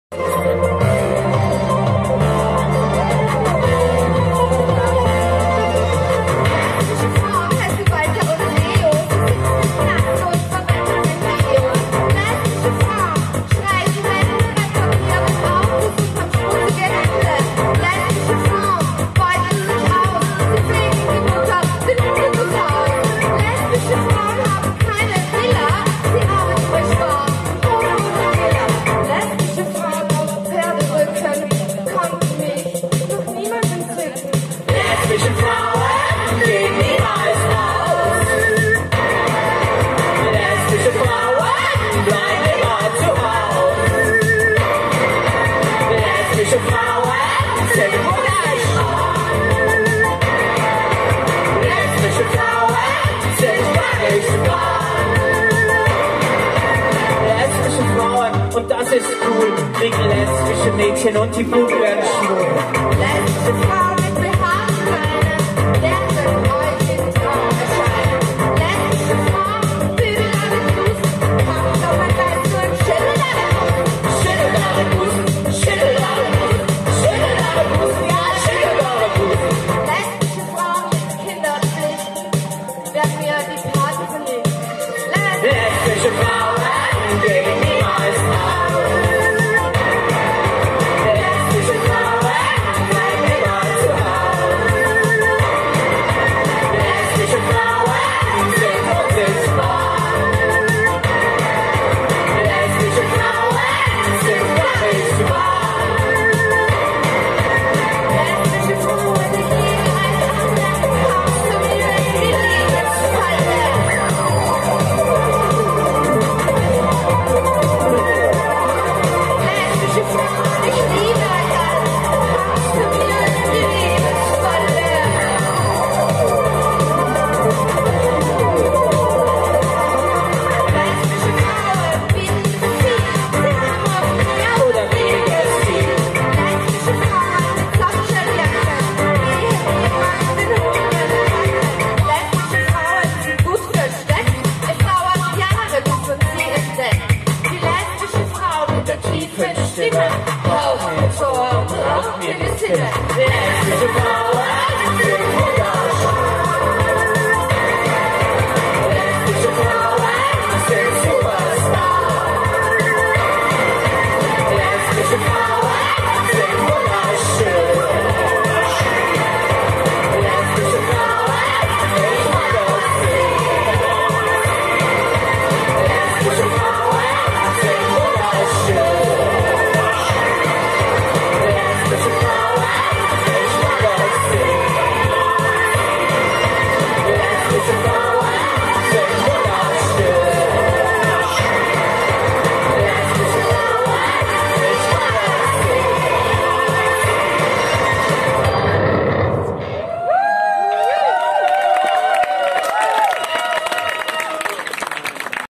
Pop:sch-2
Part of Quality Street! A performance series behind windows Bears in the Park, Kempelenpark, Vienna Favoriten